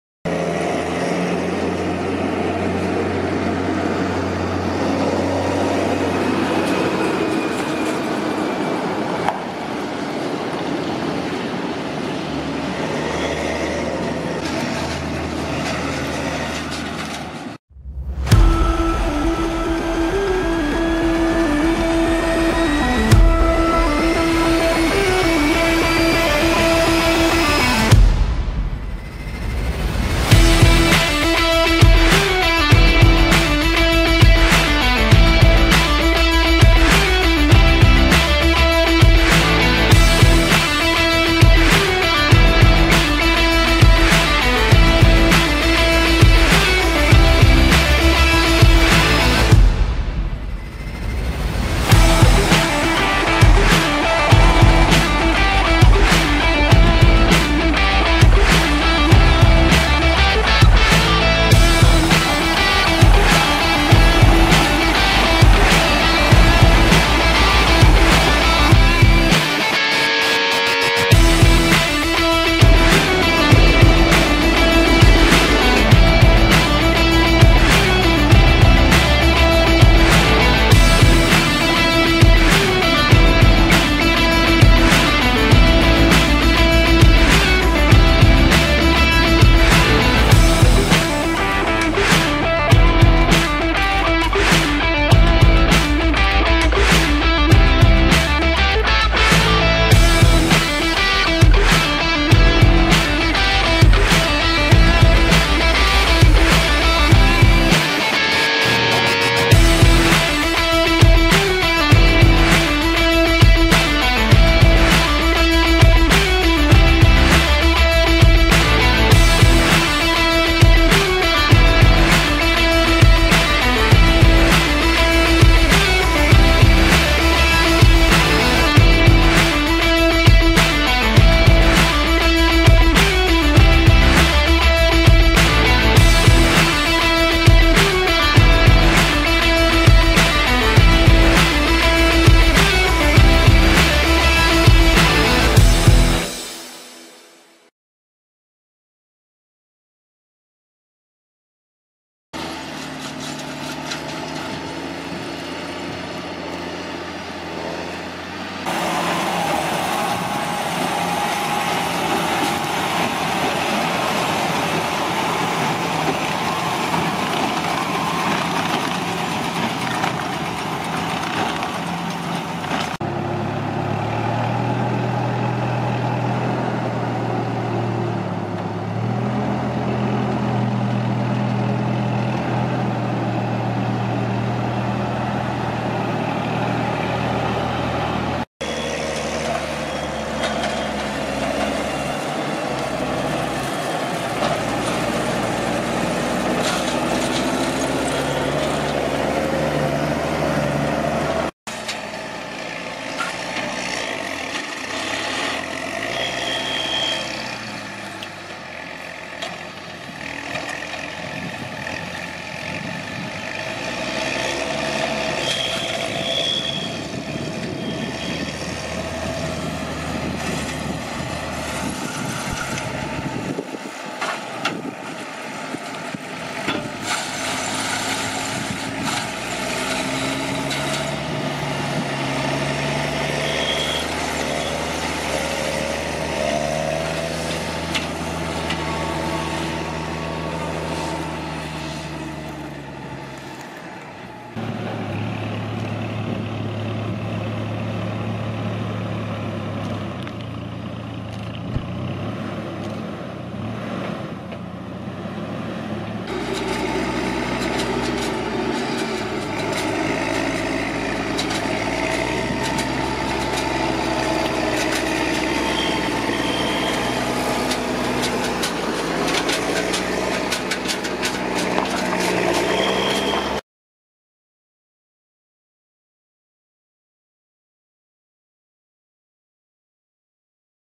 Balíkovanie slamy || New Holland T8.330 / T7060 || Dieci 40.7 // PD Pobedim 2021
Balíkovanie / Baling straw
PD Pobedim 2021

Balíkovanie New Holland T8.330 + New Holland BIGBALER 1290
Zber balíkov New Holland T7060+Arcusin FS 54.63
Ukladanie balíkov DIECI AGRI PLUS 40.7 VS EVO2
Zemedelské Služby Roman Bubenik

Strih: Sony Vegas Pro 18
Foto-video technika:
Canon EOS
Panasonic TZ
Dron DJI Mini 2
Pre viac info a foto sleduj: